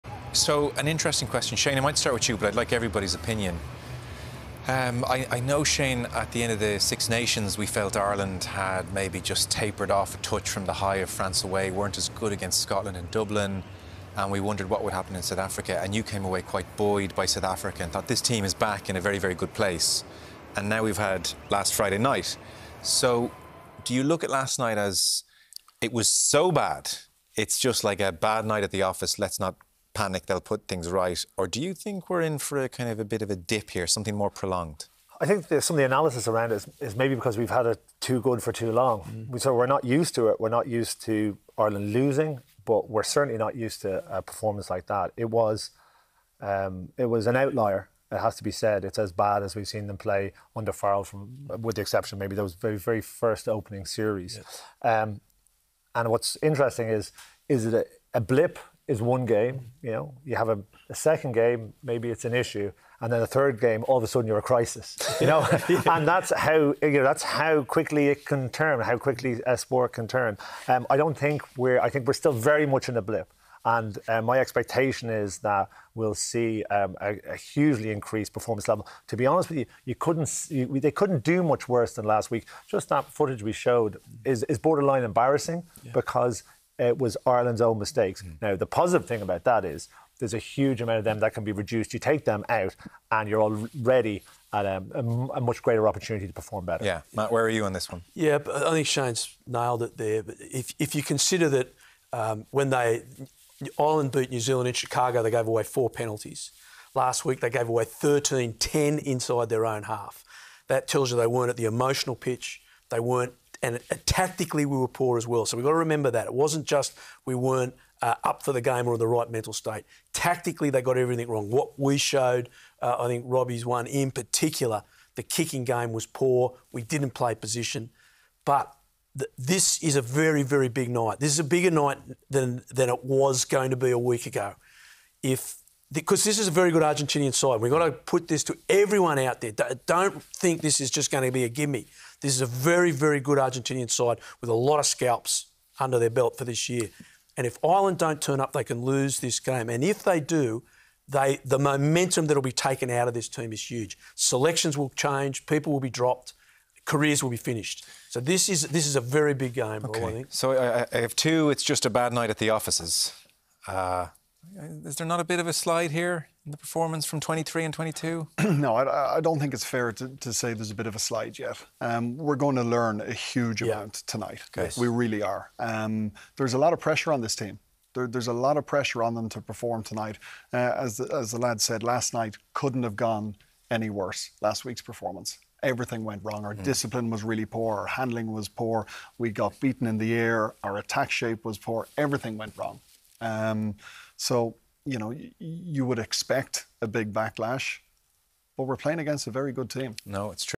Was Ireland's performance vs New Zealand cause for concern?
"A blip is 1 game. A second game, it's an issue. A third game? You're in crisis."

Are Ireland in a blip, an issue, or a crisis? 😅

Shane Horgan, Matt Williams & Rob Kearney - Athlete discuss after last week's defeat and performance.

Watch the Autumn Nations Series and the Men's, Women's and U20 Six Nations Championships live on Virgin Media Television.